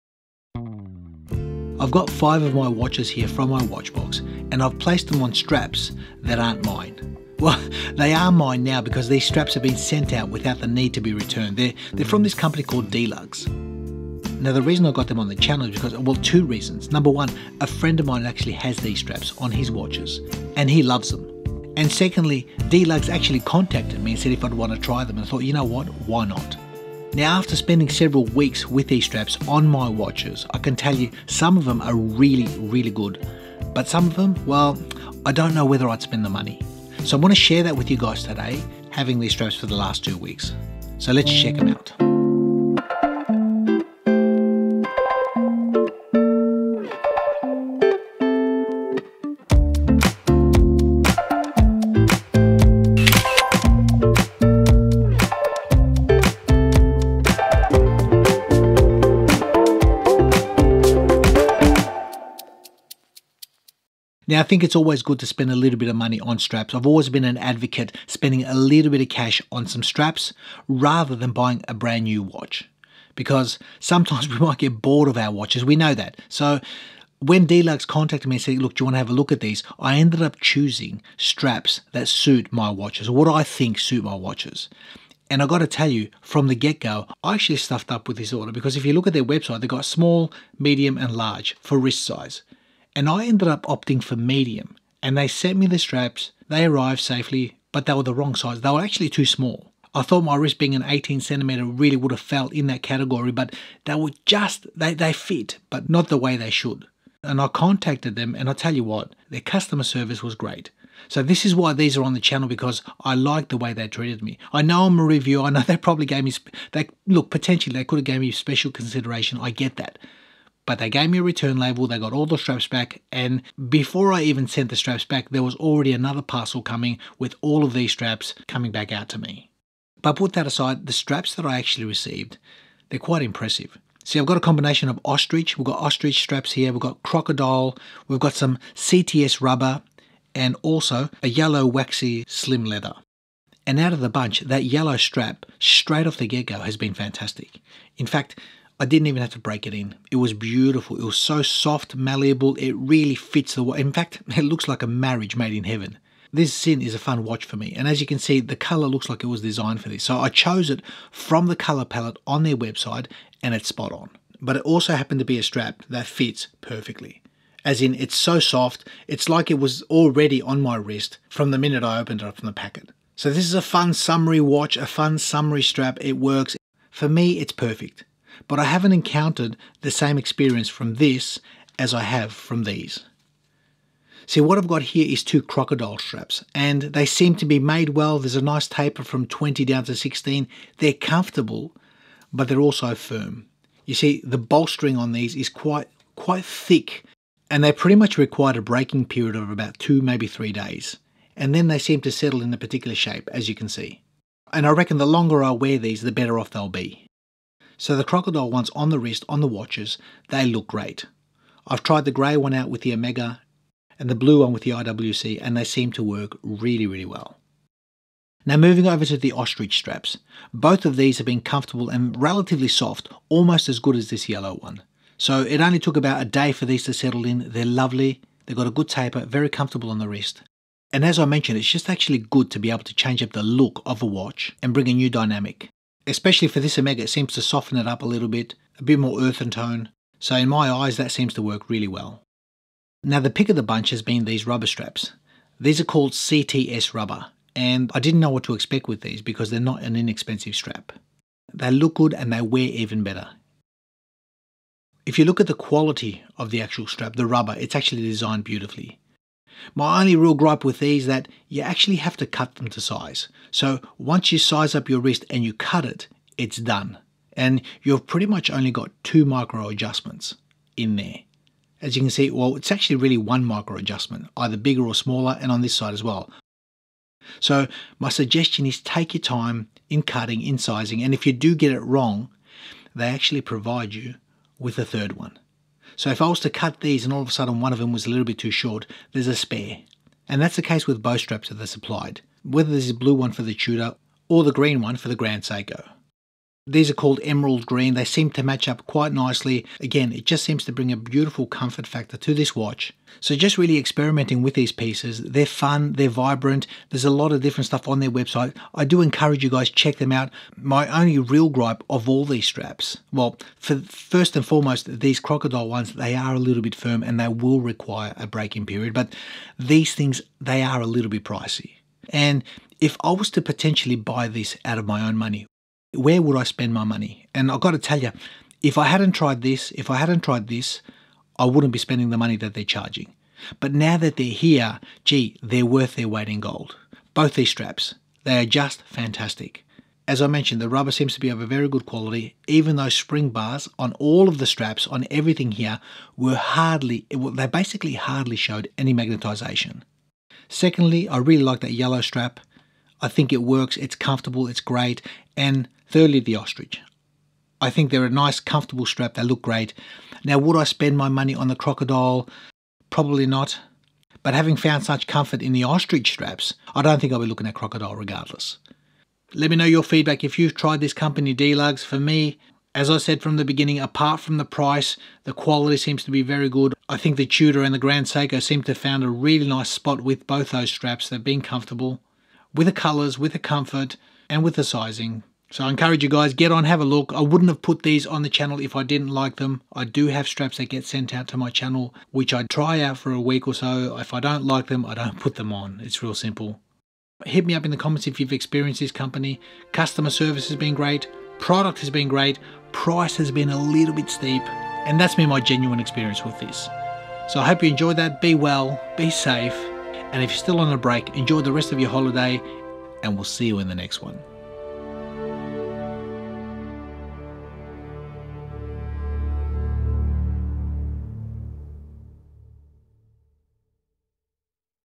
DELUGS Watch Straps
One of my friends has quite a few Delugs straps on his watches and I thought maybe I should check them out as well.
Delugs contacted me and I share my thoughts on this company and their products

Overall, very nice quality but also a tad pricey...feel free to check out the links